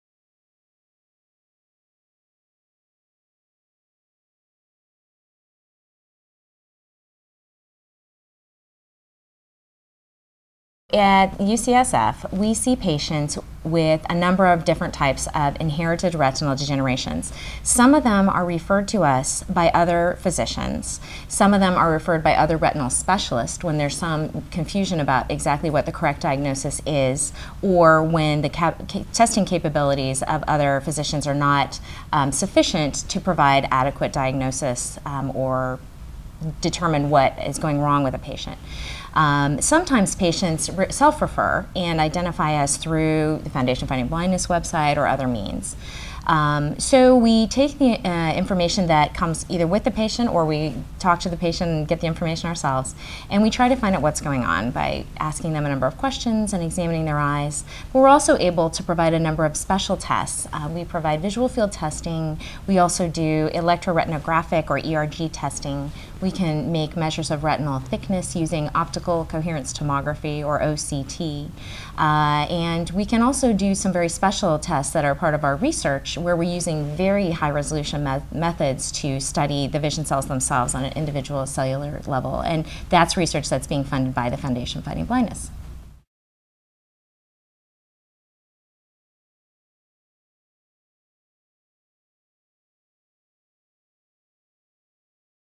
What can patients expect during an appointment at the Retinal Degeneration Center at UCSF?
| An interview with Jacque Duncan, M.D., part 2 of 12. Dr. Duncan is a researcher and professor of Clinical Ophthalmology at the University of California, San Francisco. She has expertise in the diagnosis and management of patients with retinal degenerations including age-related macular degeneration (AMD), retinitis pigmentosa, cone-rod dystrophy and Stargardt disease.